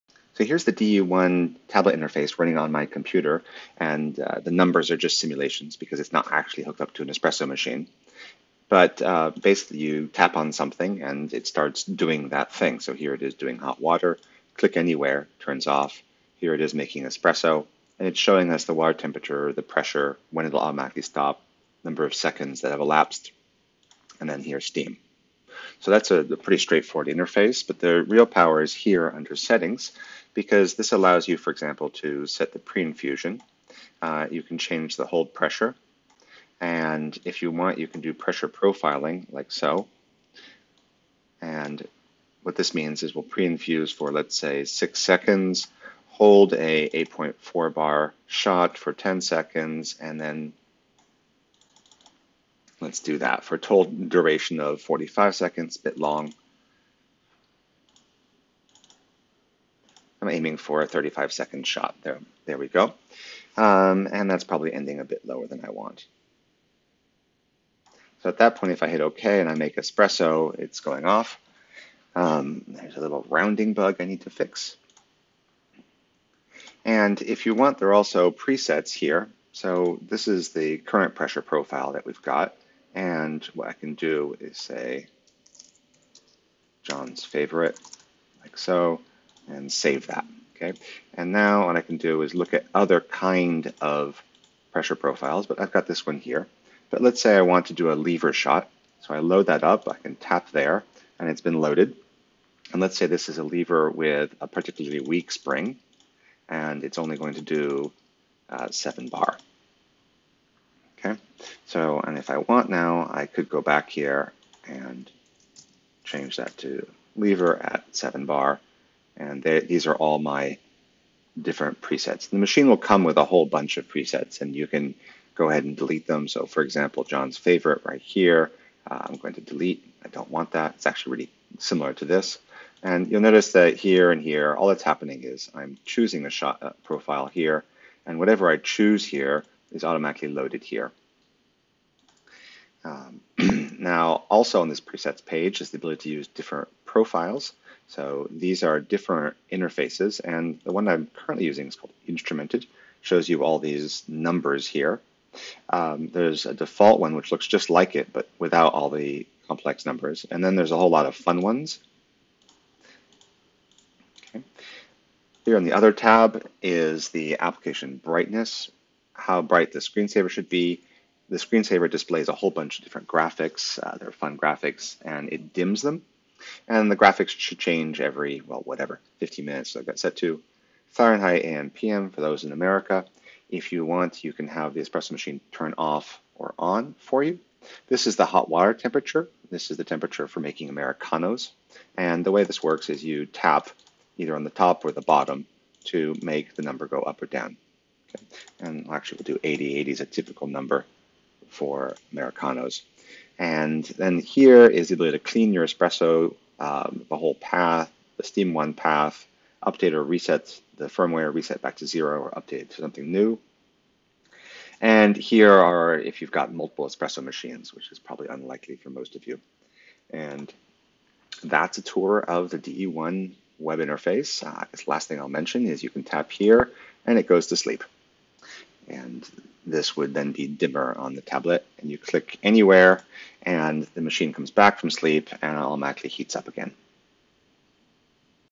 The DE1 tablet interface
I walk you through the DE1 tablet interface, showing you how the features work, how you can emulate different espresso machines, etc.